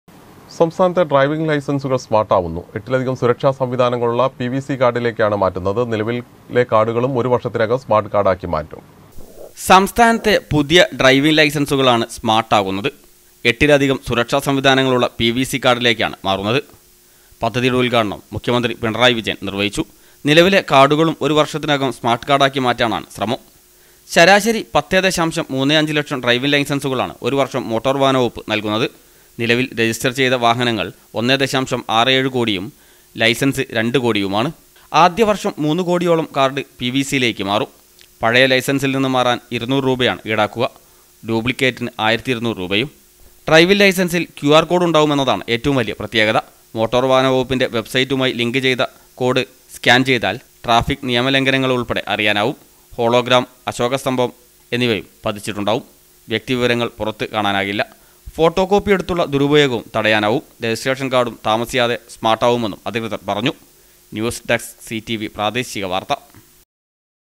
സംസ്ഥാനത്തെ ഡ്രൈവിംഗ് ലൈസൻസുകൾ സ്മാർട്ടാവുന്നു
സംസ്ഥാനത്തെ ഡ്രൈവിംഗ് ലൈസൻസുകൾ സ്മാർട്ടാവുന്നു. എട്ടിലധികം സുരക്ഷ സംവിധാനങ്ങളിള്ള പി വി സി കാർഡിലേക്കാണ് മാറുന്നത്. നിലവിലെ കാർഡുകളും ഒരു വർഷത്തിനകം സ്മാർട്ട് കാർഡാക്കി മാറ്റും